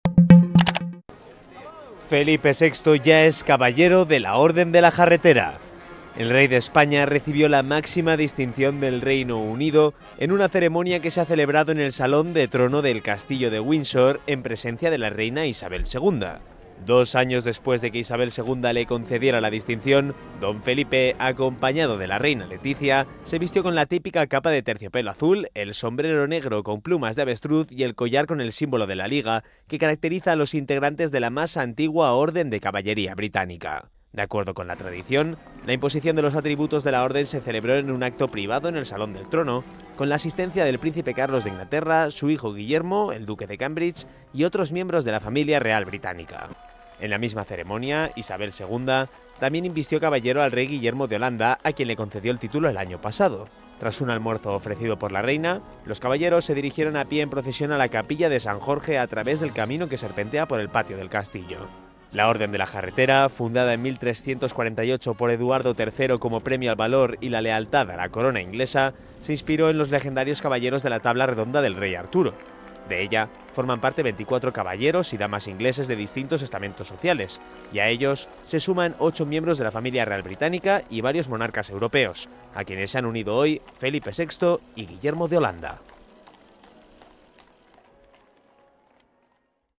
Felipe VI ya es caballero de la Orden de la Jarretera
Londres (Reino Unido), 17 jun (EFE).- (Imagen: Jesús Bartolomé) Felipe VI fue investido este mediodía nuevo caballero de la Orden de la Jarretera, la máxima distinción del Reino Unido, en una ceremonia que se ha celebrado en el Salón de Trono de Castillo de Windsor en presencia de la reina Isabel II.
CONTIENE IMAGEN POOL DE TVE.

Edición: Eugenio Ponz